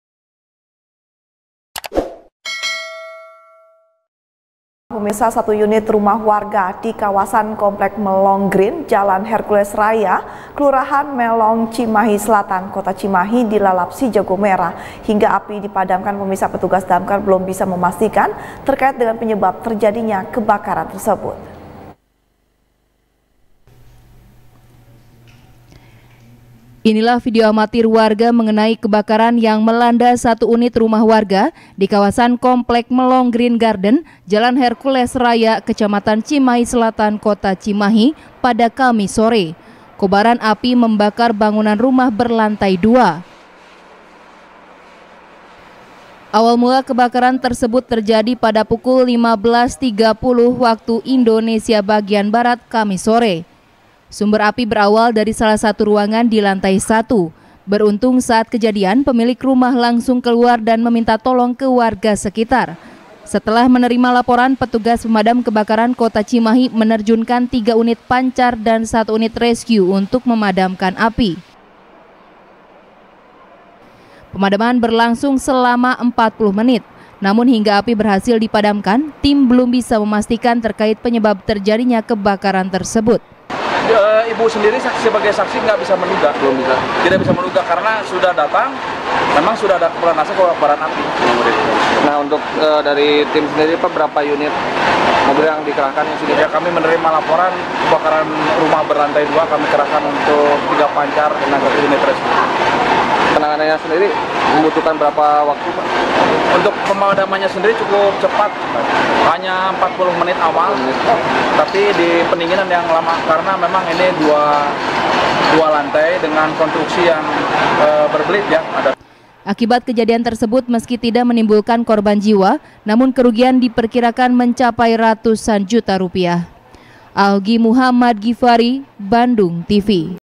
Satu Unit Rumah Warga Hangus Dilalap Si Jago Merah | SBR | BANDUNGTV
SBR | BANDUNGTV | FARHAT MUMTAZ |Satu Unit Rumah Warga,Di Kawasan Komplek Melong Green, Jalan Hercules Raya,Kelurahan Melong ,Cimahi Selatan,Kota Cimahi, Di Lalap Si Jago Merah. Hingga Api Dipadamkan, Petugas Damkar Belum Bisa Memastikan Terkait Penyebab Terjadinya Kebakaran Tersebut.